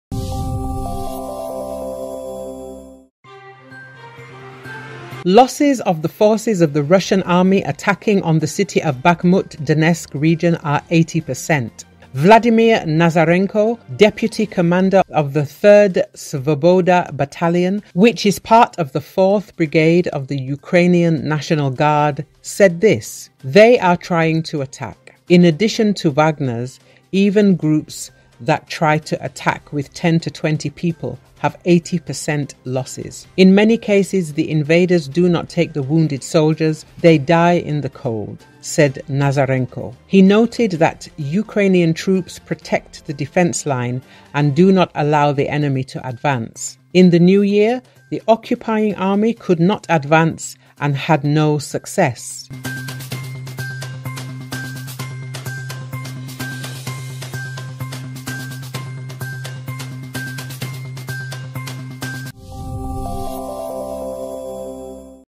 Russians lost 80% of their forces in Bakhmut, the wounded freezing to death - Ukrainian commander
Rus ordusunun Donetsk vilayətinin Baxmut şəhərində hücuma keçən qüvvələrinin itkiləri 80% təşkil edir.
Bunu telemarafonun efirində Ukrayna milli qvardiyasının 4-cü briqadasının tərkibində olan 3-cü “Svoboda” batalyonu komandirinin müavini Vladimir Nazarenko deyib.
“Onlar hücum etməyə cəhd edir. “Vaqnerçilər”dən əlavə, hətta 10-20 nəfərlə hücuma kəçməyə cəhd edən dəstələrin də itkiləri 80% olur. Bir çox hallarda işğalçılar yaralı əsgərləri götürmür, onlar da soyuqda ölür”, deyə Nazarenko bildirib.
O qeyd edib ki, Ukrayna qoşunları müdafiə xəttini qoruyur və düşmənə irəliləməyə imkan vermir. Yeni ildə işğalçı ordu irəliləyə bilməyib və heç bir uğuru olmayıb.